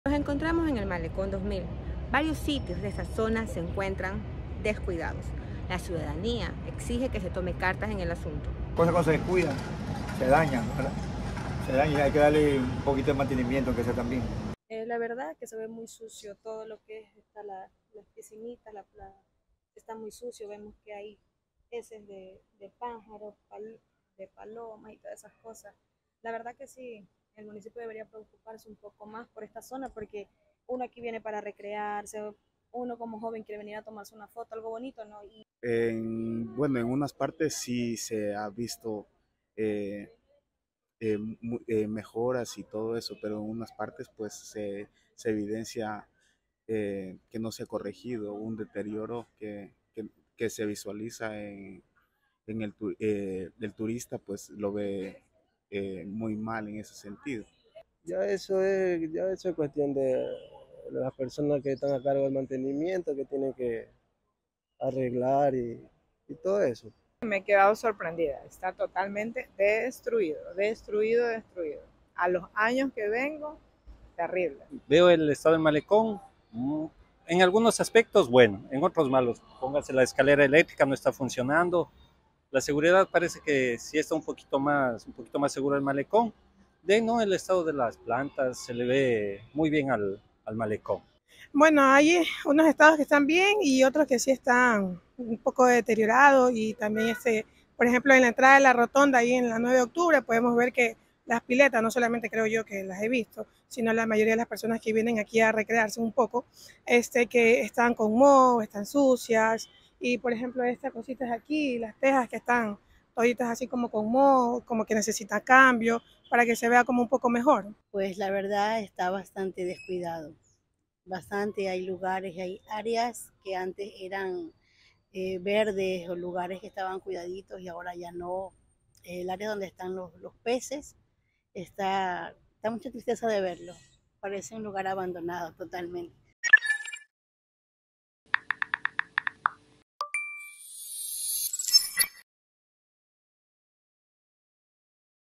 El deterioro opaca al Malecón 2000
A la ciudadanía le preocupa que el turista pierda interés en visitar este sitio atractivo ante la falta de mantenimiento. Los daños llaman la atención.